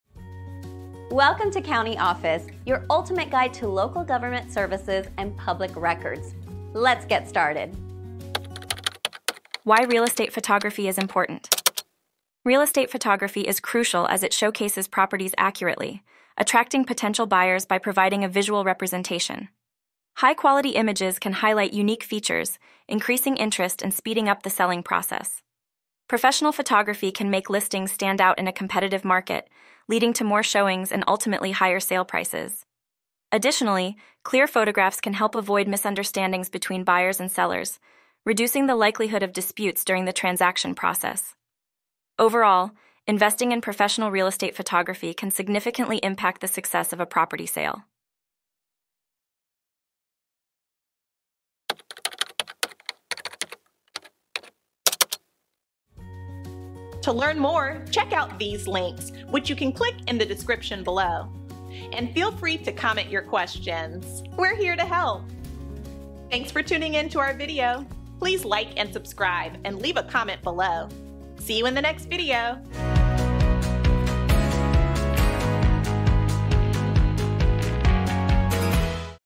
Why Real Estate Photography Is Important? - CountyOffice.org
Why Real Estate Photography Is Important? In this insightful video, we delve into the significance of real estate photography and why it plays a crucial role in showcasing properties accurately. High-quality images not only attract potential buyers but also expedite the selling process by emphasizing unique features. Professional photography elevates listings in a competitive market, resulting in increased interest, more showings, and ultimately, higher sale prices. Moreover, clear photographs can prevent misunderstandings between buyers and sellers, minimizing the chances of disputes during transactions. Investing in professional real estate photography can have a profound impact on the success of a property sale.